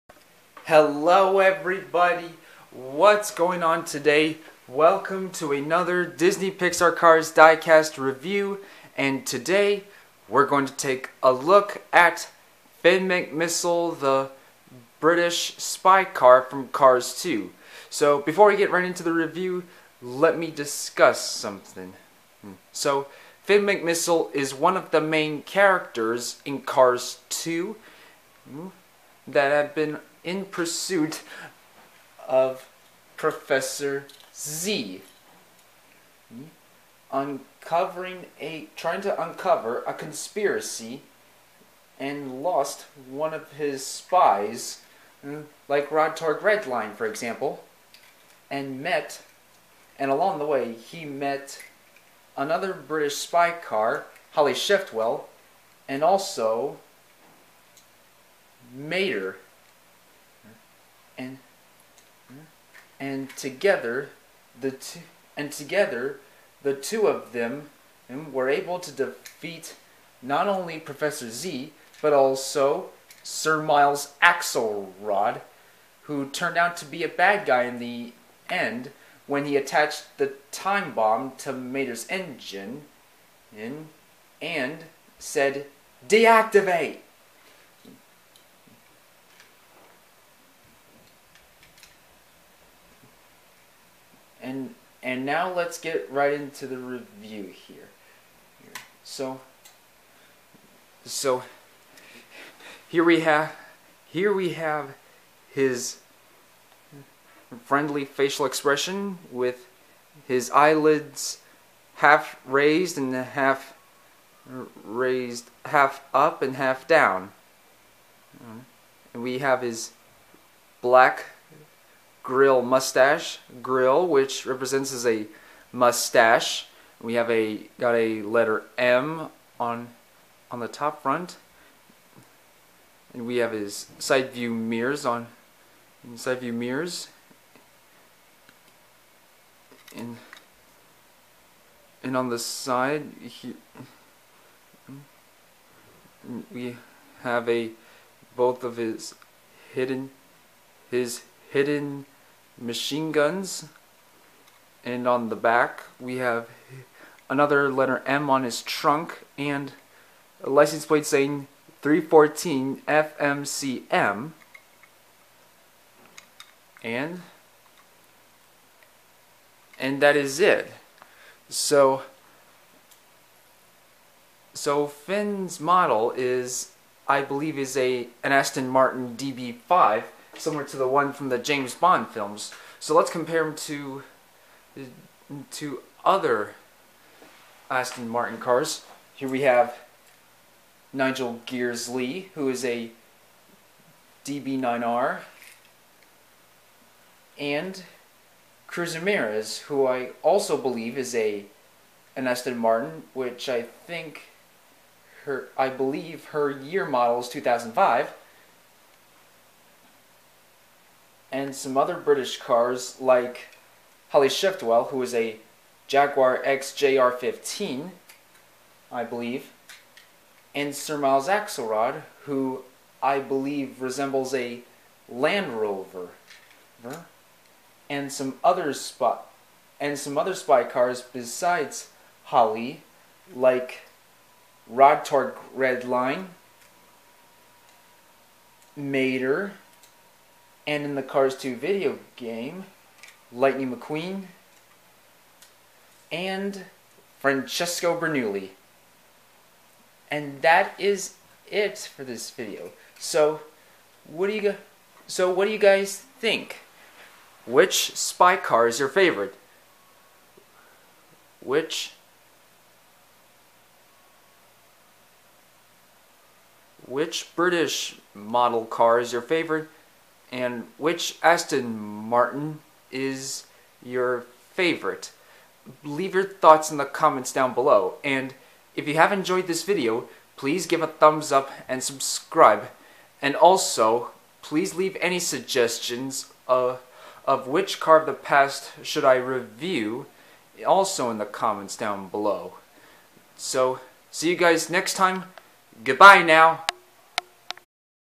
Disney Pixar Cars 2 Finn McMissile (British spy car) diecast
Hello everybody & today, we're gonna take a look at Finn McMissile the british spy car from Cars 2. Finn McMissile is one of my favorite characters from Cars 2 & I decided to give the Cars 2 diecasts a bit more spotlight.
Cars belongs to Disney/Pixar.
DB5 is a trademark of Aston Martin.